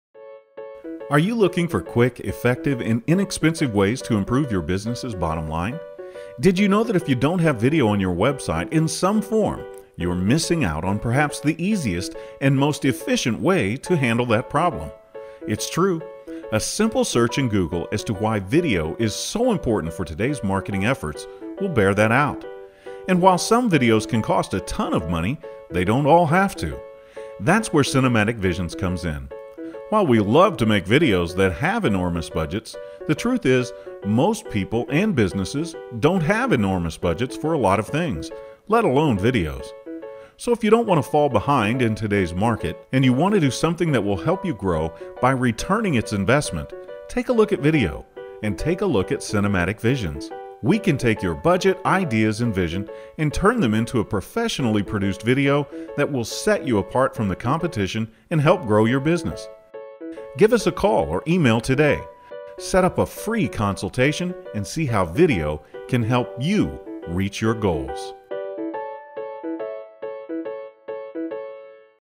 Why is Video important for your business?
Why is video important for your business?  Because people will watch a video quicker than they will read something.  They get to know the real you - essentially shaking hands with you through the screen.  In addition, video builds trust, curiosity, confidence and even amusement!  Last but not least - it is a fantastic way to move up the rankings in search engines like Google,  Yahoo and Bing.  What are you waiting for?  Give video a try - call Cinematic Visions, video produciton services in Kansas City, Missouri, today.

Produced by Cinematic Visions
20 NW Chipman Road, #200
Lee's Summit, MO 64063